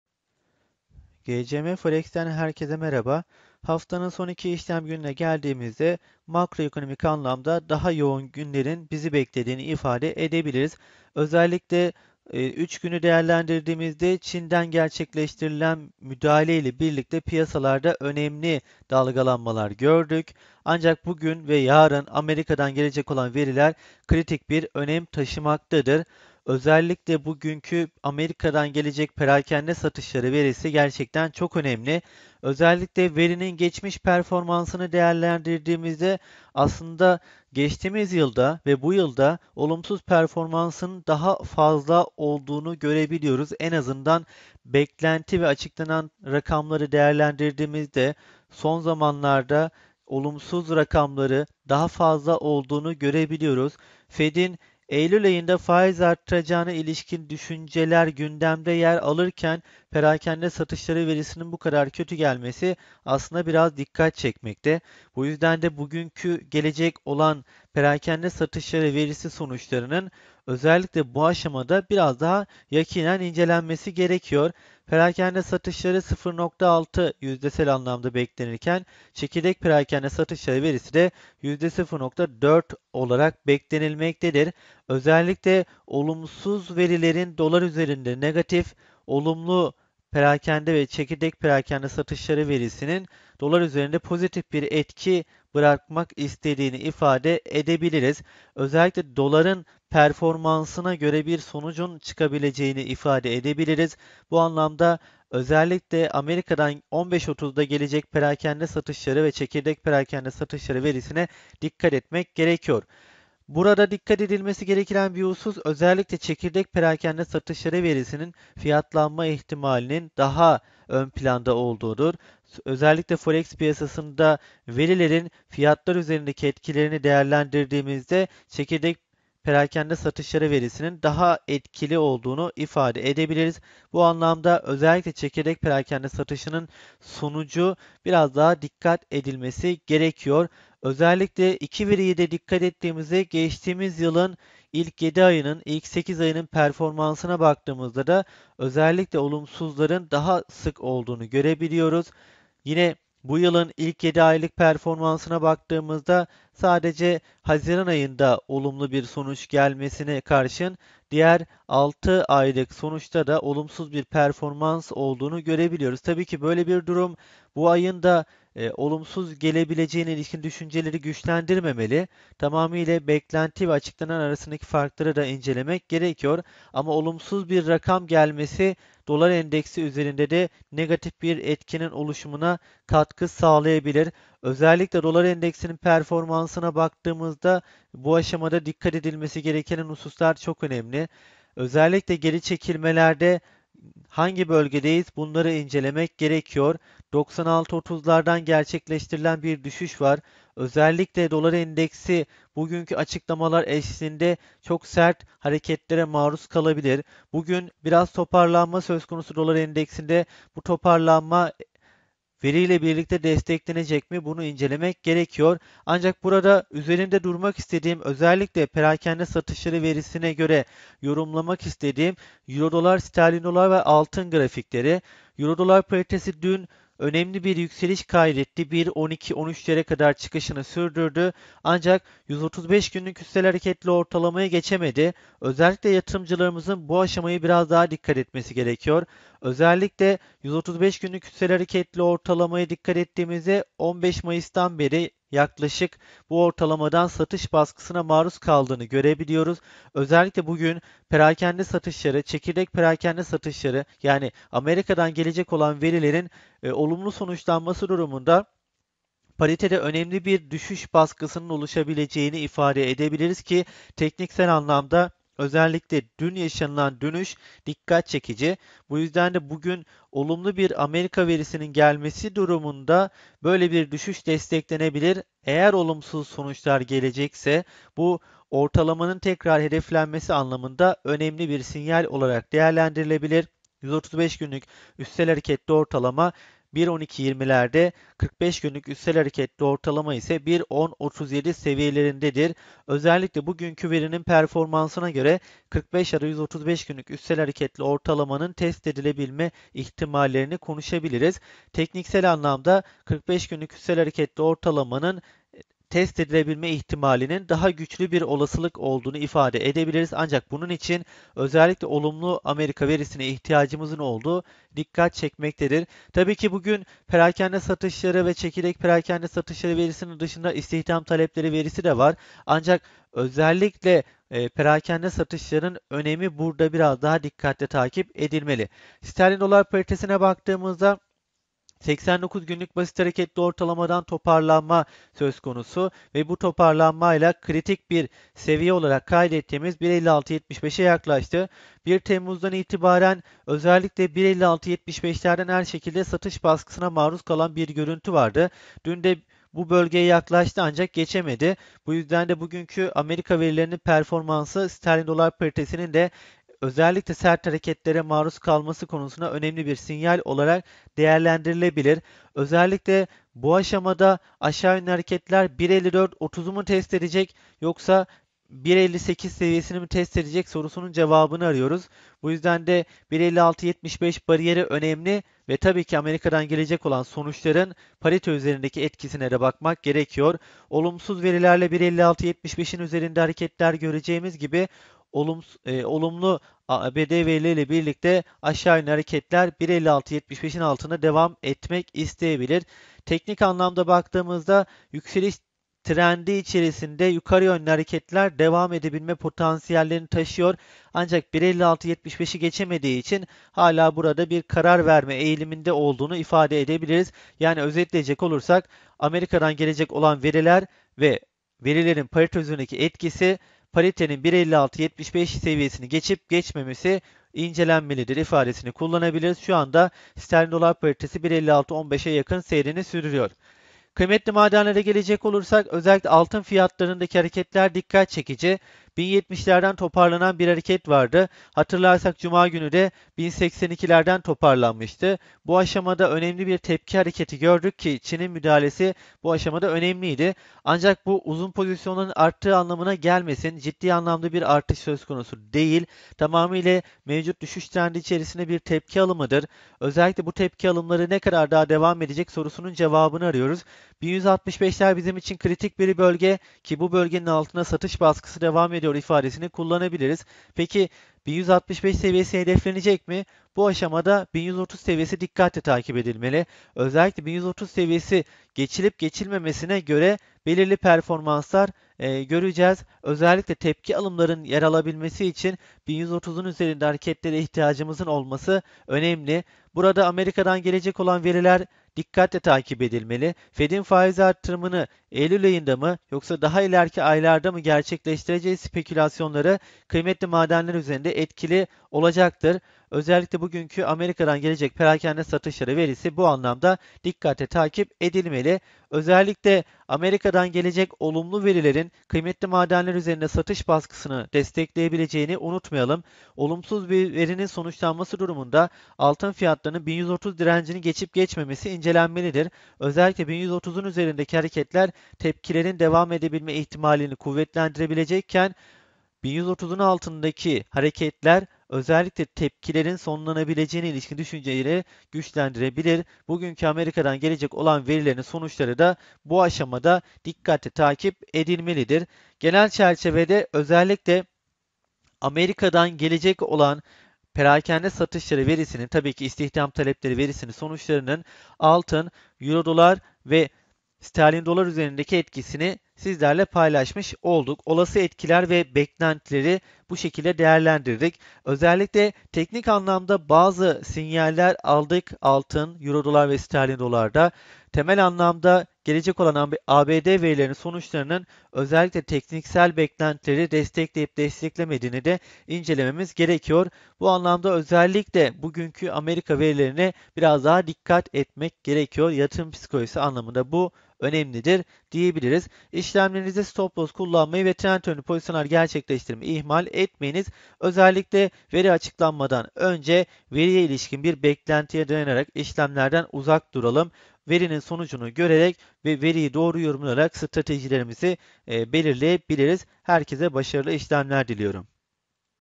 Perakende Satışları Verisine Dikkat / Kudret AYYILDIR / 13.08.2015
ABD'den gelecek olan Perakende Satışları, Çekirdek Perakende Satışları ve İstihdam Talepleri verisi eşliğinde bugün Dolar Endeksi, EURUSD, GBPUSD ve Altın'da sert hareketler görülebilir. Temel ve Teknik analiz eşliğinde ilgili finansal varlıklar ve ekonomik takvim verilerinin yorumlanmasını ilgili videoda izleyebilirsiniz.